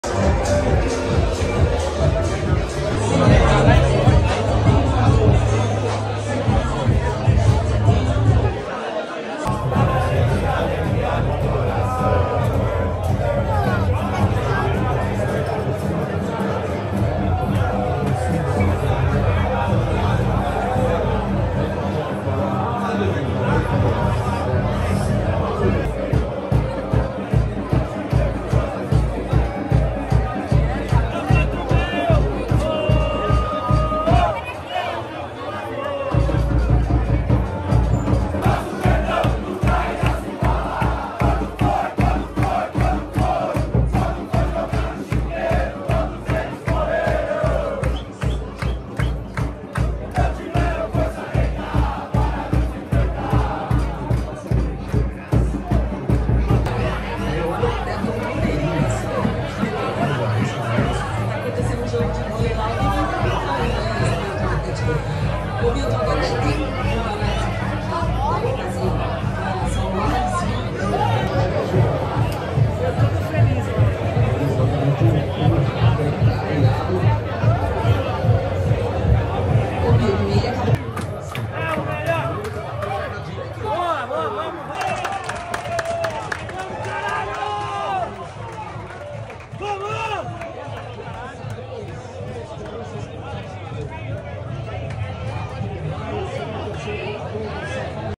FUTEBOL EM DUBLIN!?
Em Dublin os torcedores do Palmeiras tem um cantinho pra matar a saudade da energia do estádio!

Esse espaço fica localizado no andar subterrâneo do Pub The Buskers, na região do Temple Bar.

São várias TVs espalhadas pelo espaço que é gigante, e nos ambientes internos é possível acompanhar jogos de campeonatos europeus.

Além disso, o espaço conta com mesas de sinuca e vários outros jogos para você se divertir enquanto acompanha os jogos e “entorna” uma pint! 🍺